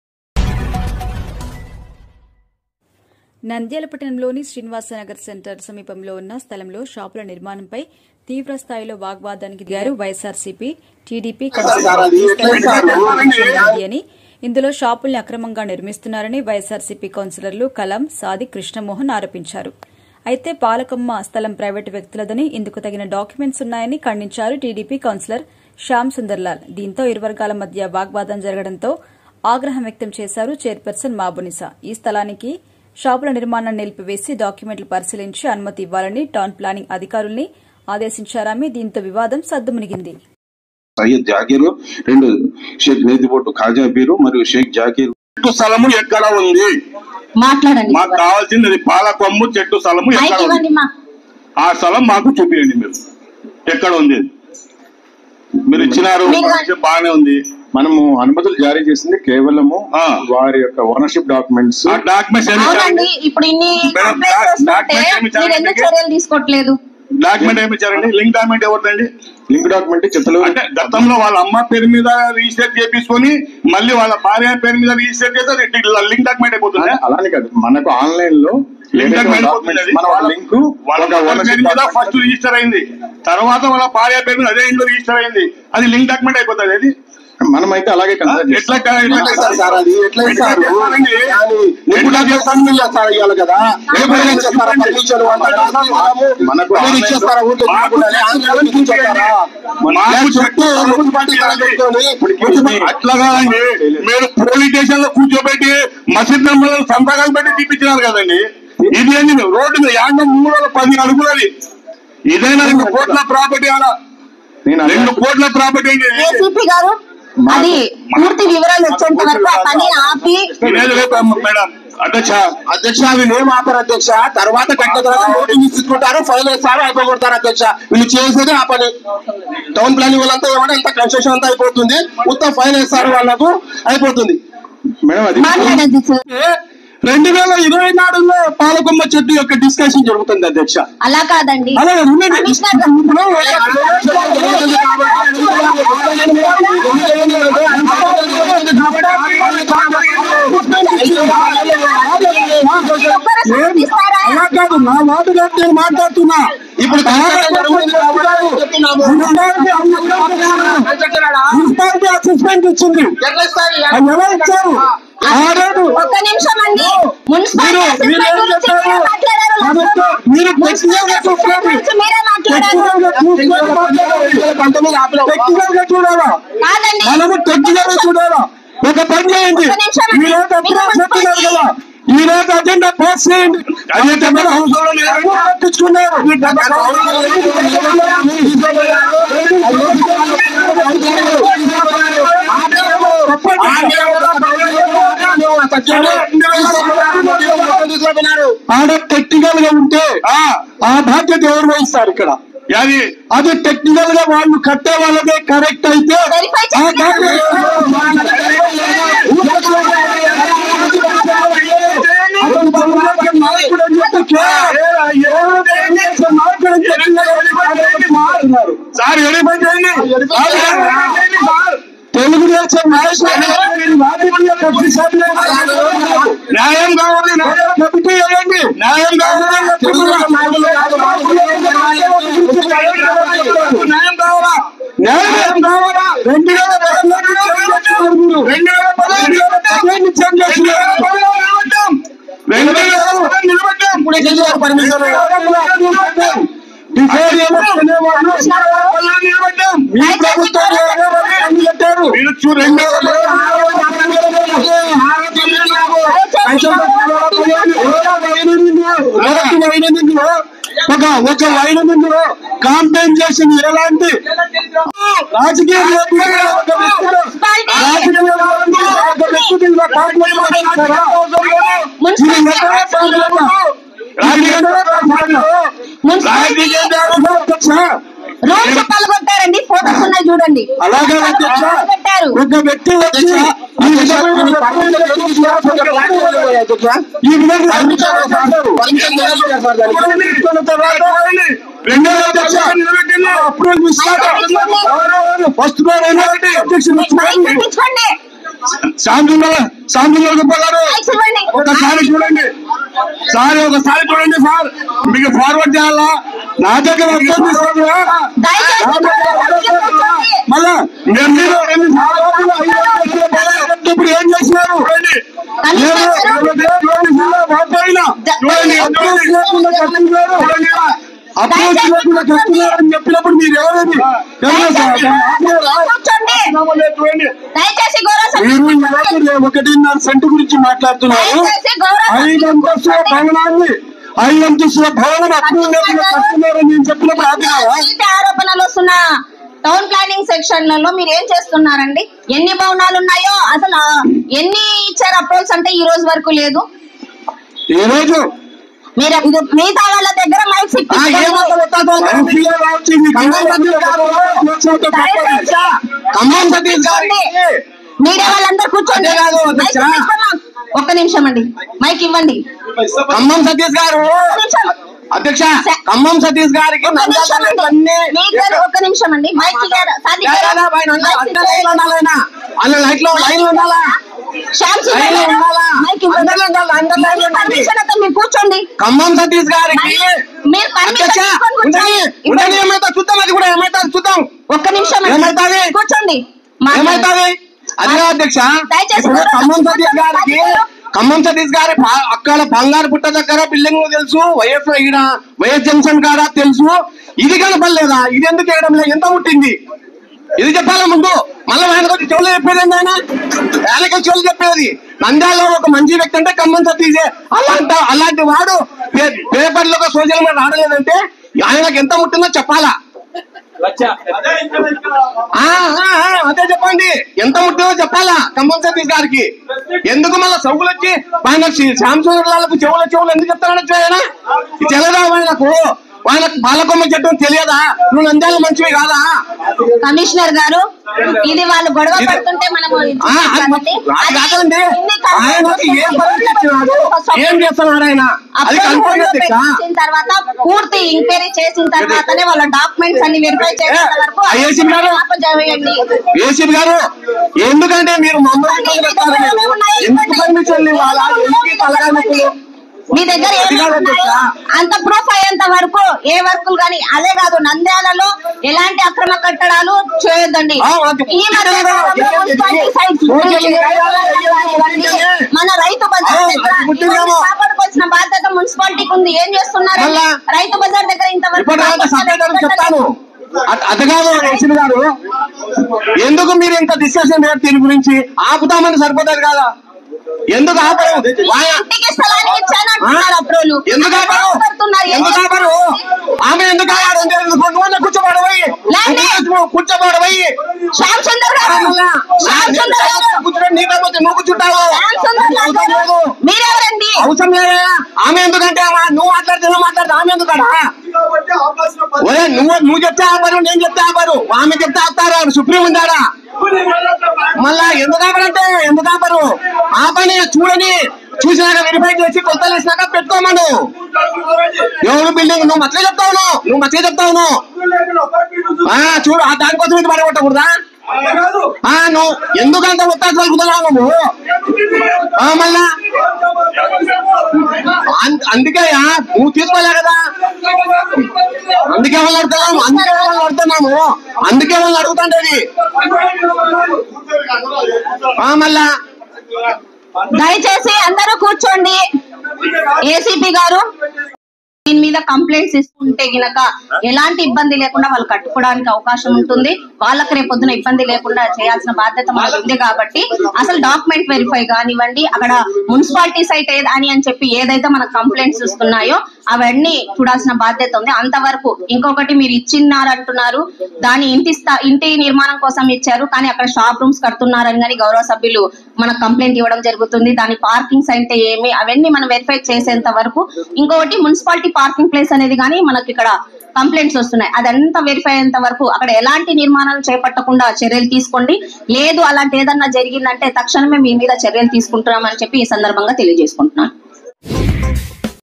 మున్సిపల్ కౌన్సిల్ లో దుమారం పాలకొమ్ము చెట్టు ఎక్కడ మున్సిపల్ స్థలం ఎక్కడ.
Nandyal news 9 ---పాలకొమ్ము చెట్టు ఎక్కడ మున్సిపల్ స్థలం ఎక్కడ
పాలకొమ్మ చెట్టు స్థలం అక్రమణపై మున్సిపల్ కౌన్సిల్ లో దుమారం